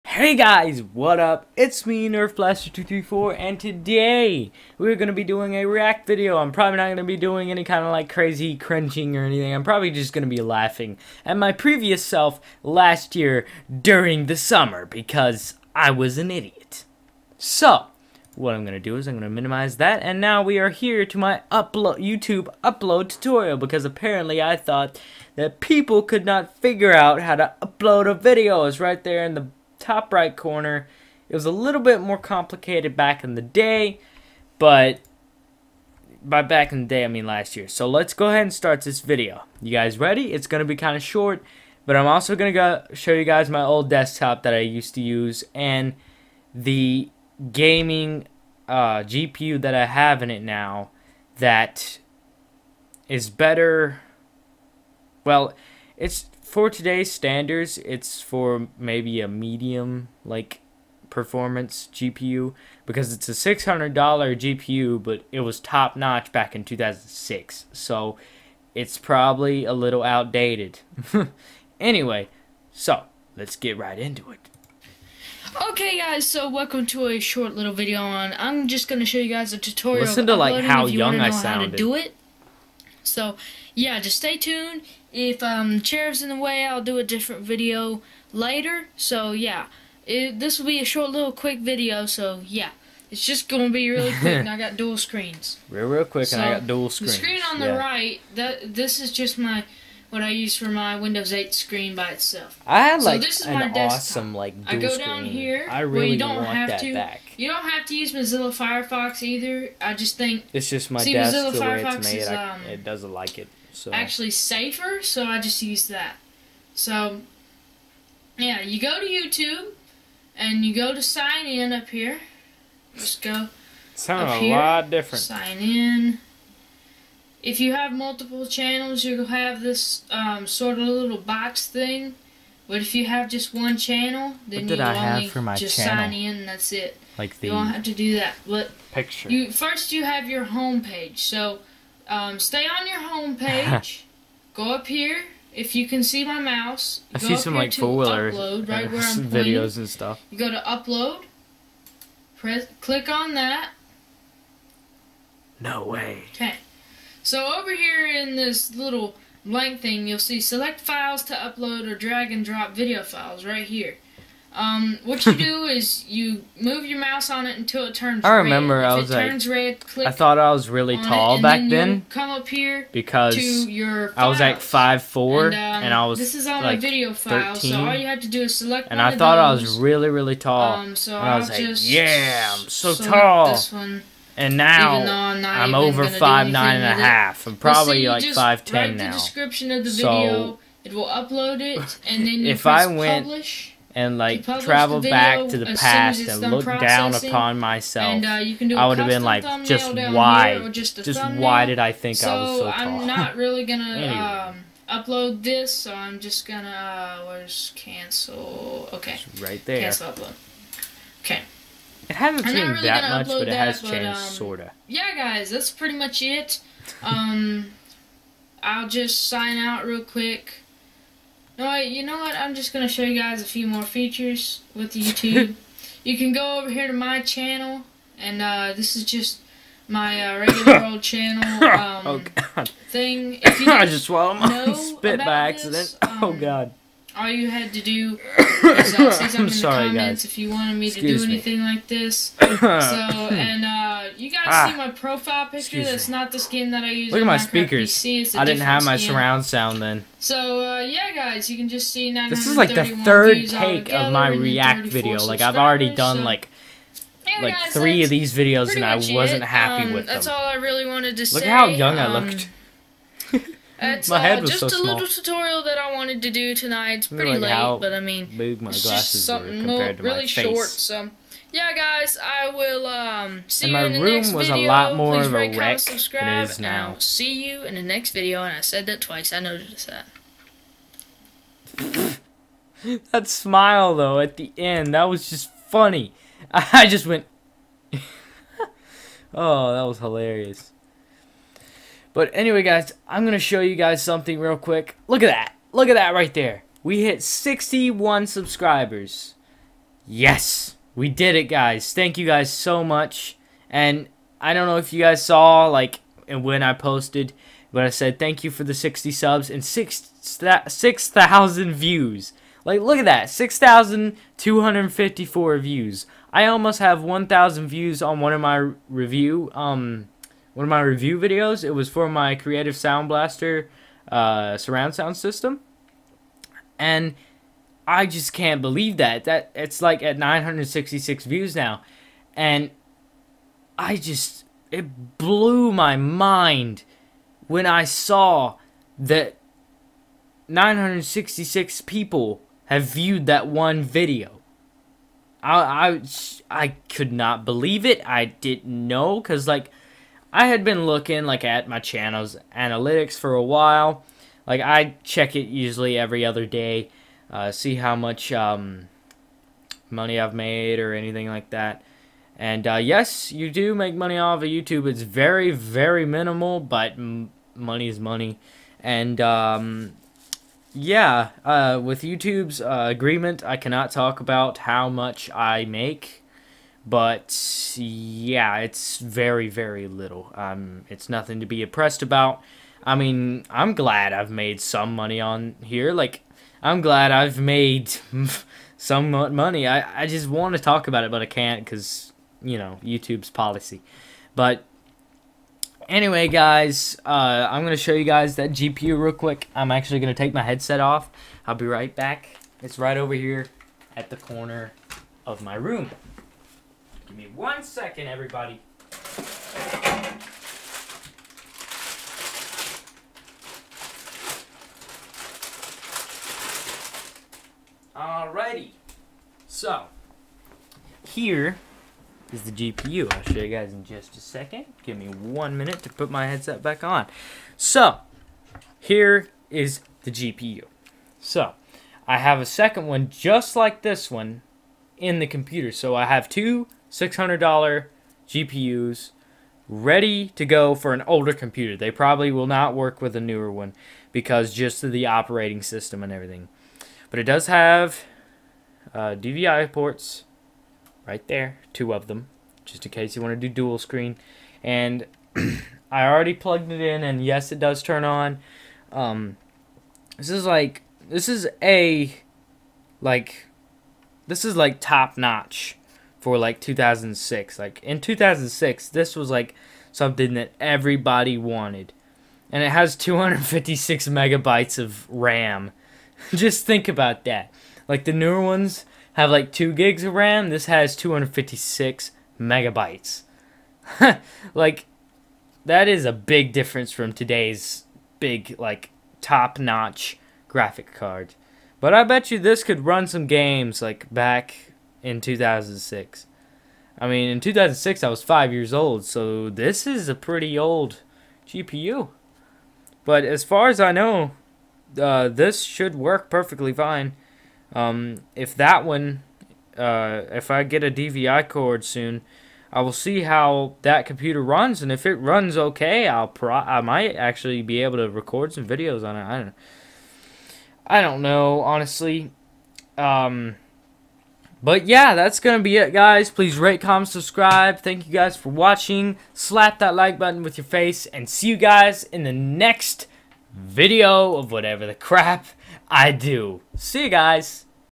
I react to my uploading tutorial
Hey guys in this video we visit the past and look at how different and awkward I was. I hope you enjoy!!! :)